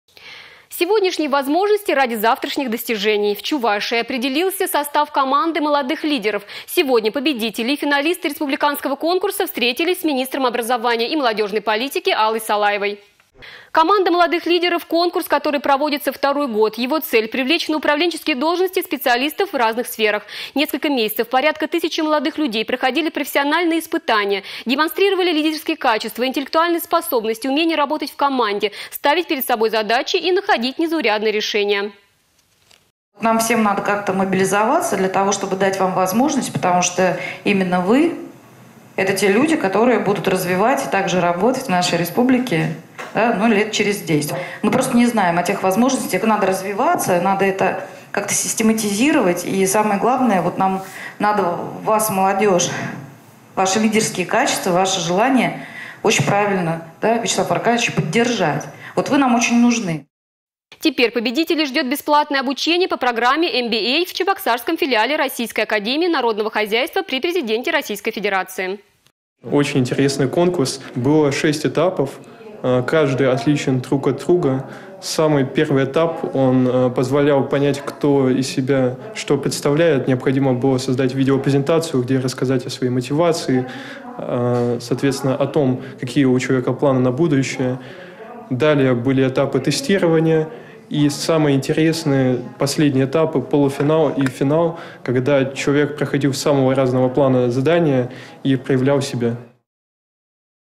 В Чувашии определился состав команды молодых лидеров
Сегодняшние возможности ради завтрашних достижений. В Чувашии определился состав команды молодых лидеров. Сегодня победители и финалисты республиканского конкурса встретились с министром образования и молодежной политики Аллой Салаевой.